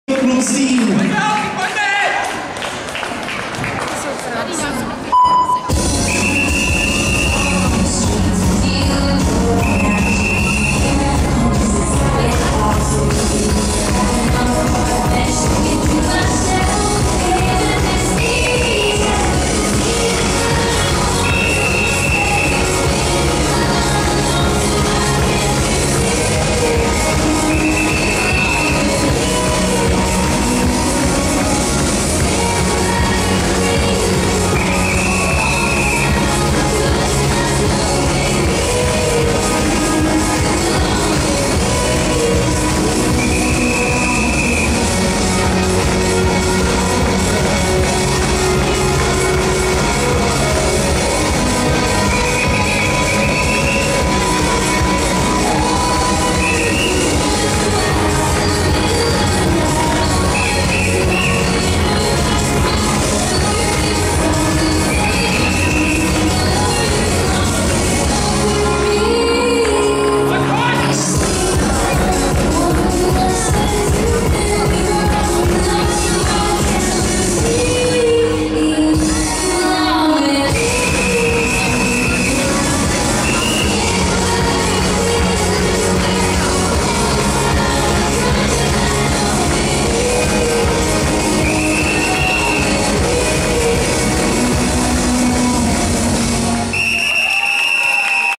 Aerobik klub Zlín - III.VT 17. 5. 2015 Varnsdorf
Elenka Smetanová, Barunka Večerková, Barunka Vodvárková, Barunka Kostelecká, Verunka Nováková, Sofinka Šimková a Agátka Marušáková - základní kolo 1. místo